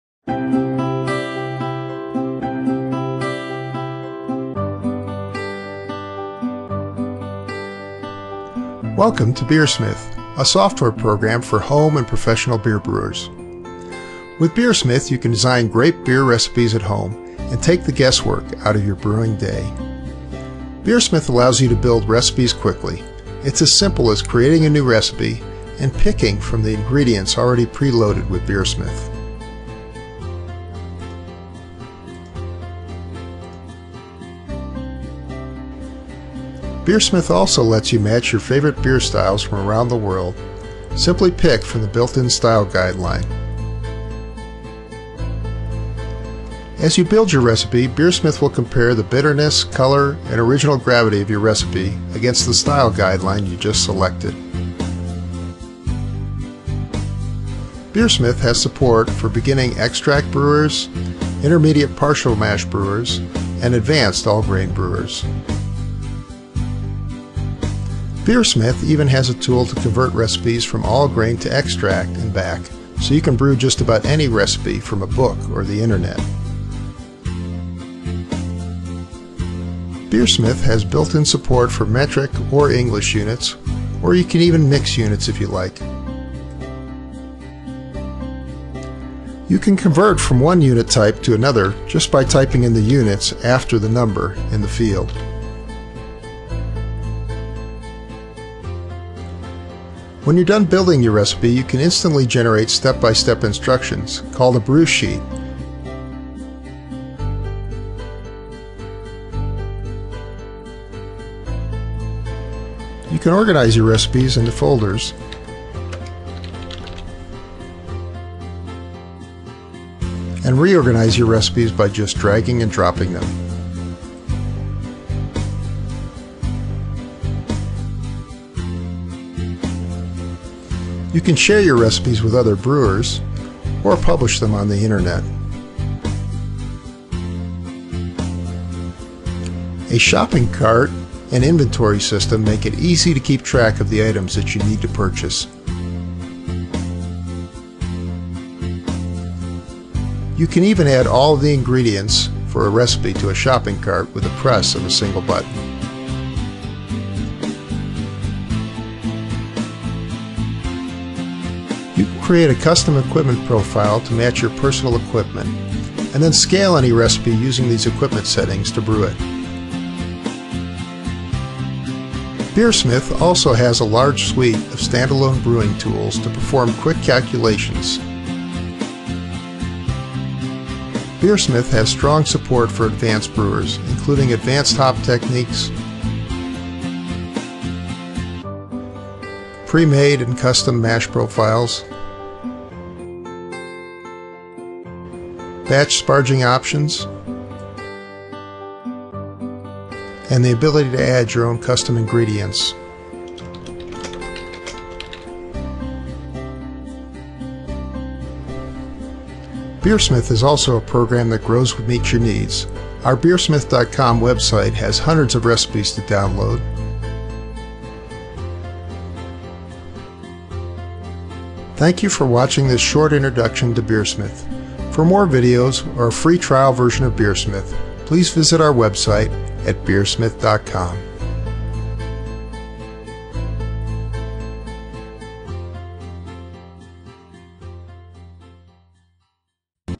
BeerSmith Home Brewing Software Overview
A video overview of the BeerSmith Home Brewing software from BeerSmith.com for making great beer recipes at home.  BeerSmith is a tool for homebrewers to formulate beer recipes, perform various brewing calculations and organize your recipes.  for the full size version high quality version of this video.